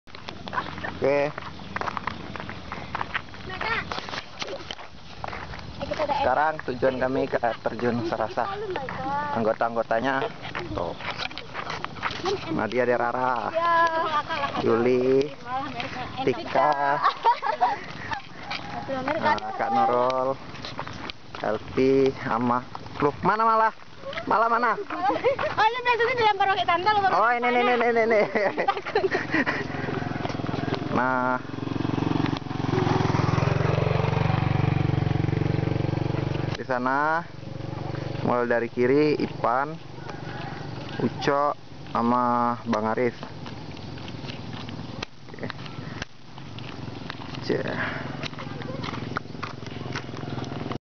Air Terjun Sarasah (Awal Perjalanan)
Perjalanan kelompok mahasiswa Pendidikan Luar Biasa / PLB, UNP
Padang. Ke Air terjun sarasah di Gadut.